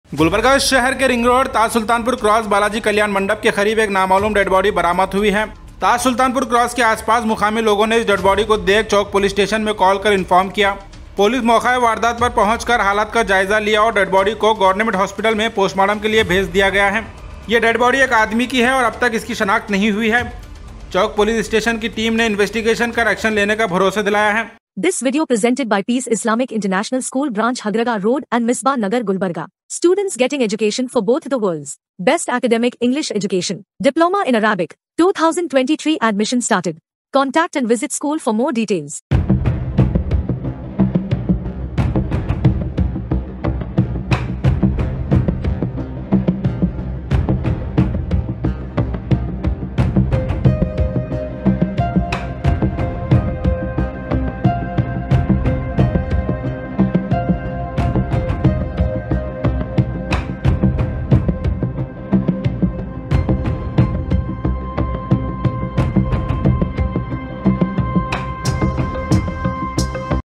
Unidentified Body Discovered Near Balaji Complex on Taj Sultanpur Road, Chowk Police Investigate
Gulbarga:- Unidentified Body Discovered Near Balaji Complex on Taj Sultanpur Road, Chowk Police Investigate